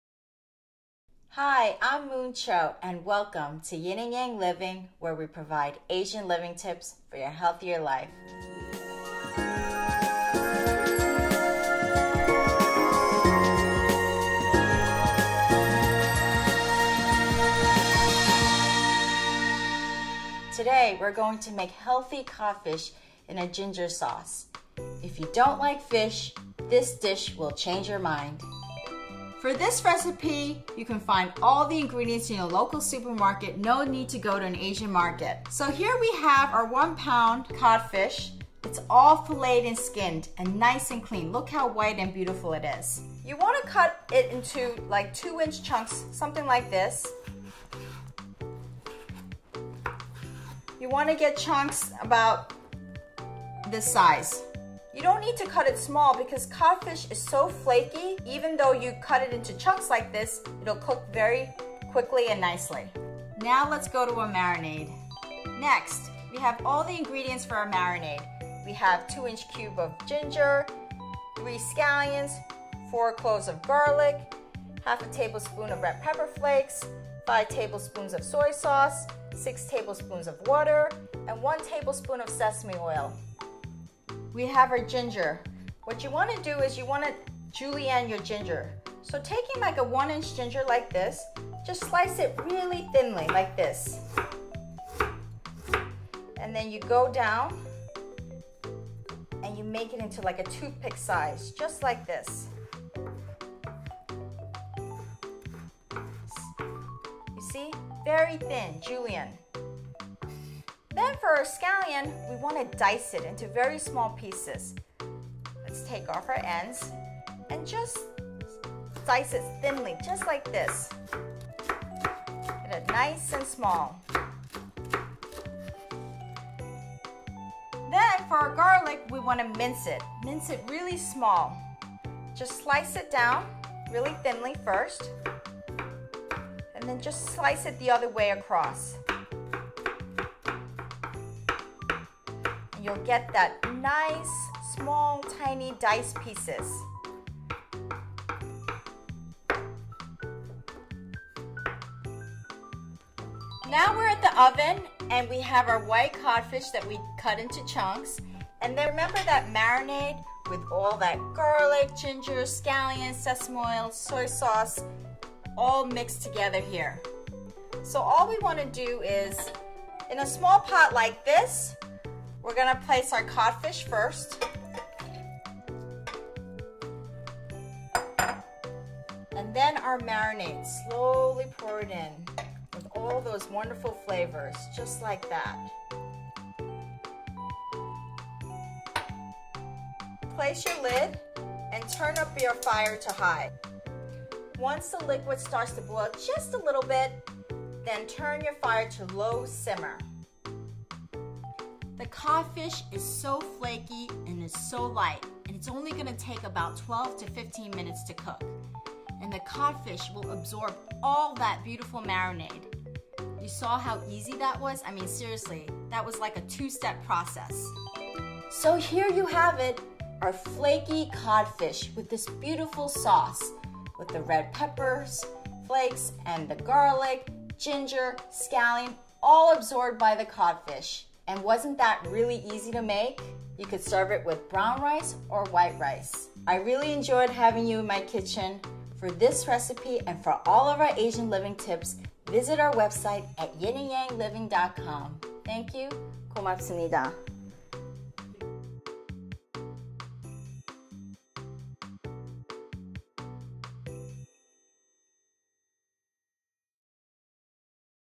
Easy Cod Fish in Soy Sauce and Ginger Marinade Asian Recipe
Learn how to make this very easy Asian fish recipe: a light flaky cod fish in a savory soy sauce with ginger, garlic and scallions. So light, flaky and healthy Asian fish dish. For this recipe, you can find all the ingredients in your local supermarket, no need to go to an Asian market.